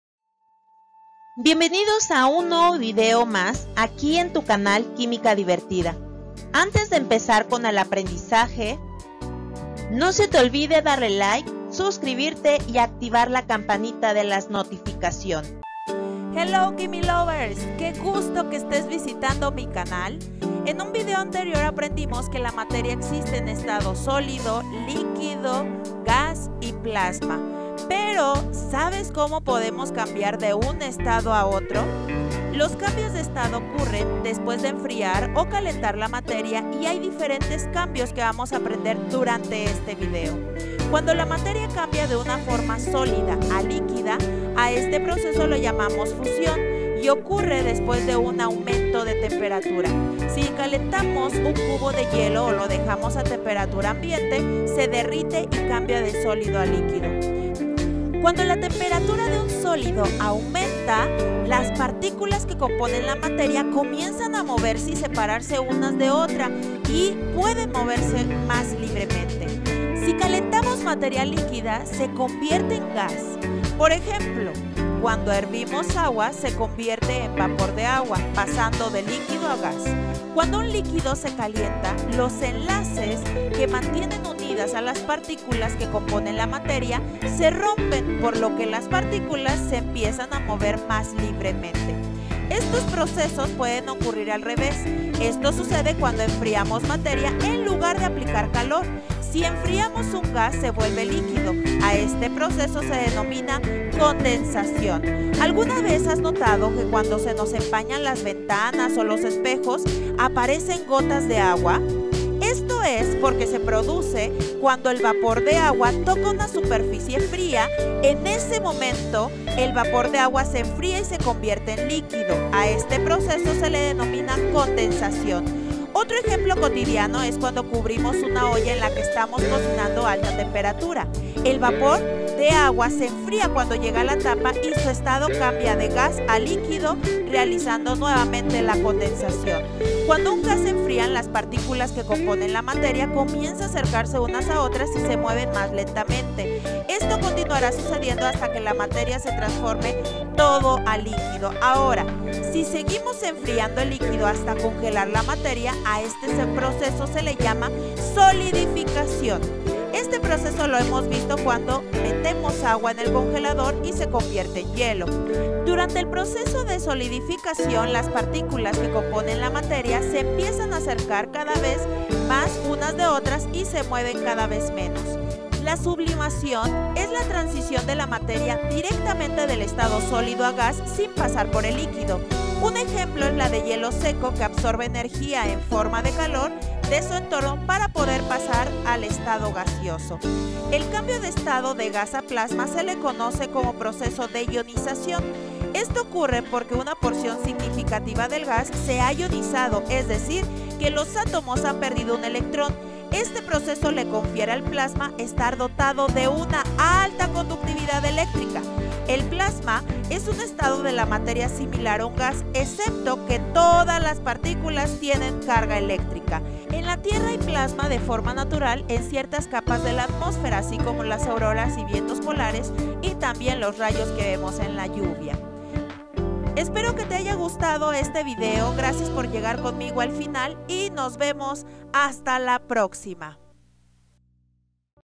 Cambios de estados de la materia: fusión, vaporización, condensación, solidificación, y más
"¡Sumérgete en el fascinante mundo de los cambios de estado de la materia con este emocionante video! Desde la fusión y la vaporización hasta la condensación y la solidificación, exploraremos los diferentes procesos que experimenta la materia en función de la temperatura y la presión. A través de explicaciones claras y ejemplos prácticos, aprenderás cómo se llevan a cabo estos cambios de estado y cómo afectan las propiedades físicas de los materiales. Además, descubrirás cómo se utilizan estos conocimientos en la vida cotidiana, desde la preparación de alimentos hasta la tecnología de refrigeración. ¡Prepárate para una aventura educativa en el maravilloso mundo de la química y la física de los estados de la materia!"

0:00 Bienvenida
0:22 Estados de la materia
0:36 Por qué ocurren los cambios de estado
0:41 proceso de fusión
0:52 ejemplo de fusión
1:10 vaporización
1:15 ejemplo de vaporización
1:38 condensación
1:46 ejemplo de condensación
2:06 segundo ejemplo de condensación
2:34 solidificación
2:41 ejemplo de solidificación
2:56 sublimación
3:03 ejemplo de sublimación
3:11 ionización
3:30 qué es el plasma
3:37 ejemplos de ionización